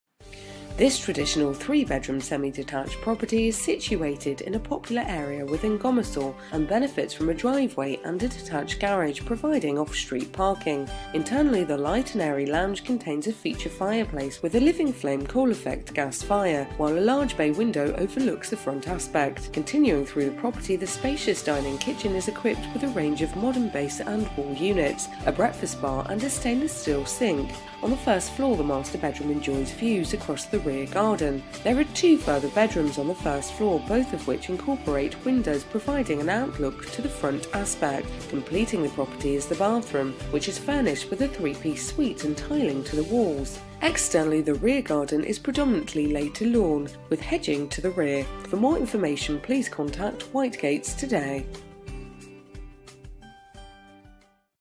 Woodlands Road, Gomersal, Cleckheaton, West Yorkshire
This traditional bay fronted semi detached property with THREE BEDROOMS and a GARAGE is priced to sell and is situated in the popular area of Gomersal with good access to local towns and schools and is just a short drive away from the motorway network links. With gas fired central heating and UPVC double glazing the property briefly comprises; entrance hall, spacious fitted dining kitchen, light and airy lounge with bay window, three bedrooms and a modern bathroom with three piece white suite. There is a paved and planted garden to the front, driveway leading to a detached garage to the side and a lawned garden with paved patio areas to the rear.